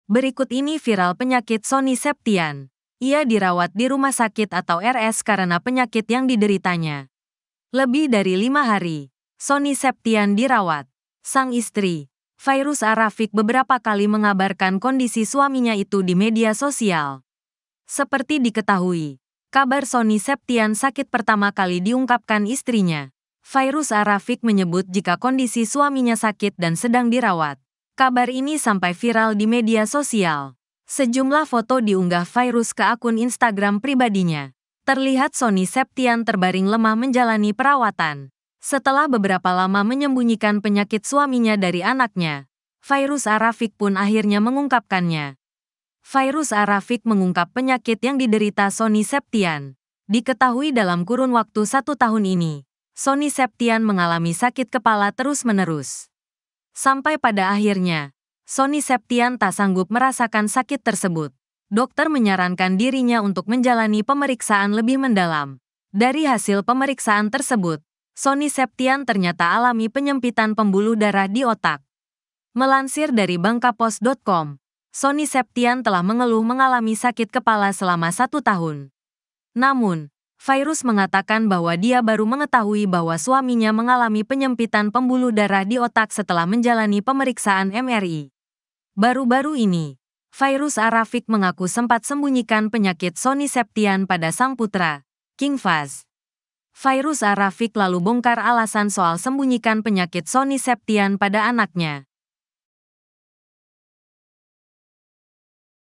Penyakit Sonny Septian, Fairuz A Rafiq Ungkap Suami Alami Penyempitan Pembuluh Darah VoD
Ia dirawat di rumah sakit atau RS karena penyakit yang dideritanya.
Lebih dari lima hari, Sonny Septian dirawat.

Program: -
Host: -
Editor Video: ZAKY